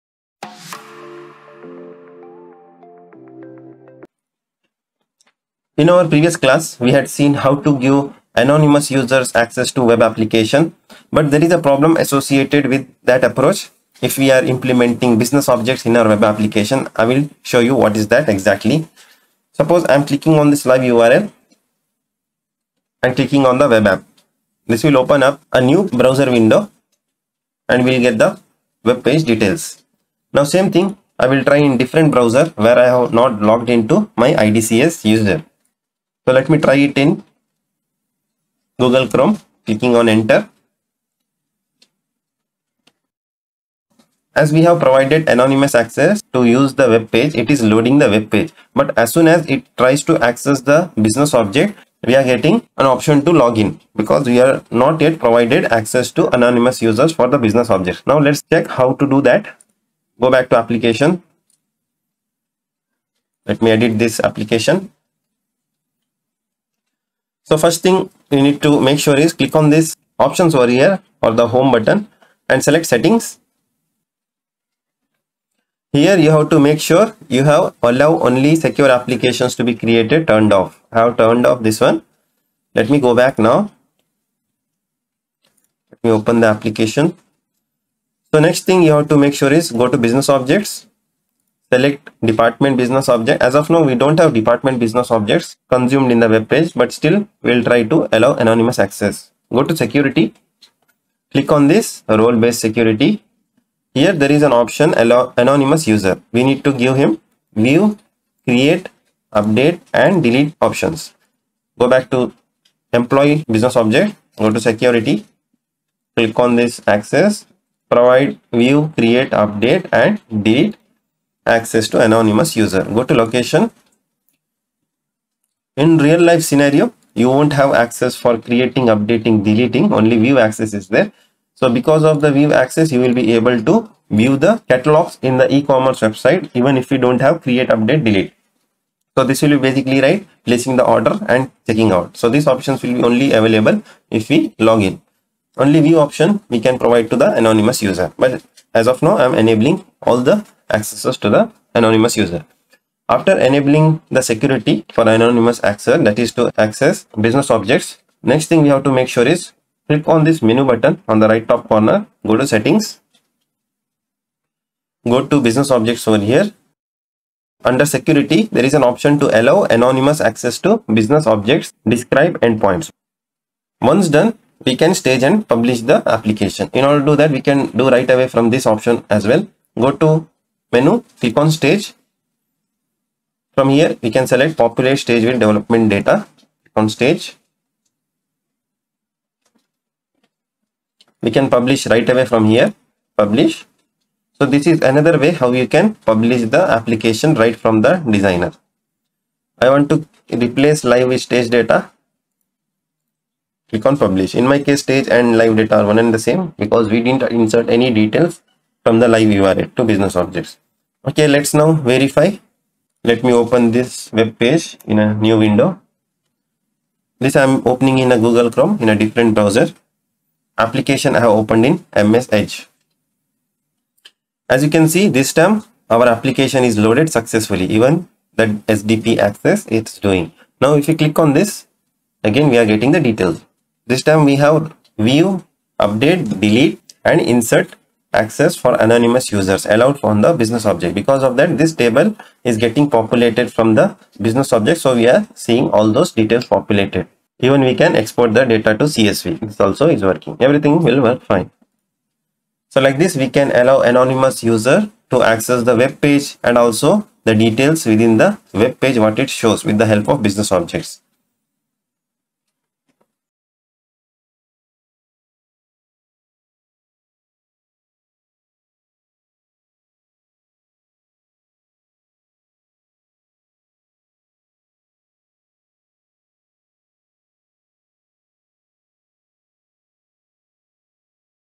How to secure Business Objects with role based security in Oracle Visual Builder (VBCS) | Anonymous
With this video we will learn  How to secure Business Objects with role based security in Oracle Visual Builder (VBCS) | Role-based security with allow anonymous users example

Click on above link for Udemy course on Visual Builder (VBCS)

0:00:03 - What you'll learn from this class
0:00:22 - Problem walkthrough with allow anonymous user example
0:01:08 - Turn off allow only secure applications to be created (only for allow anonymous users only)
0:01:38 - Enable role based security for business objects
0:03:35 - Test and validate the changes done